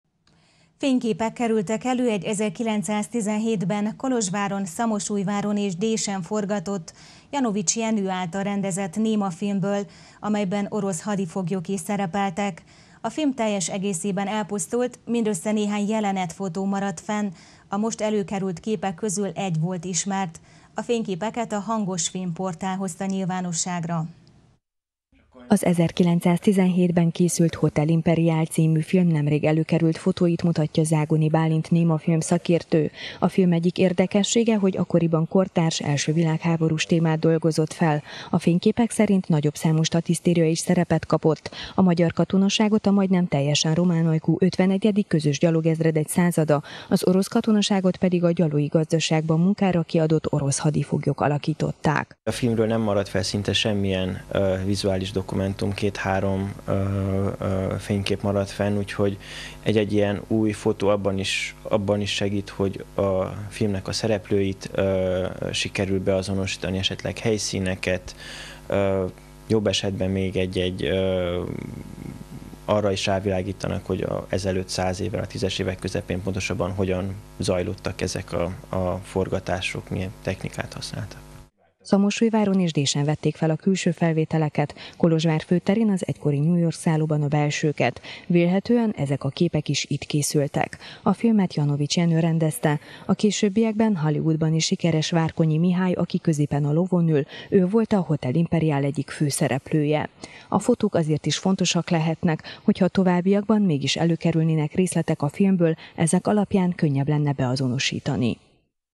Képek száz éves magyar filmből – Erdélyi Magyar Televízió
Fényképek kerültek elő egy 1917-ben Kolozsváron, Szamosújváron és Désen forgatott, Janovics Jenő által rendezett némafilmből, amelyben orosz hadifoglyok is szerepeltek. A film teljes egészében elpusztult, mindössze néhány jelenetfotó maradt fent belőle, a most előkerült képek közül egy volt ismert. A fényképeket a Hangosfilm portál hozta nyilvánosságra.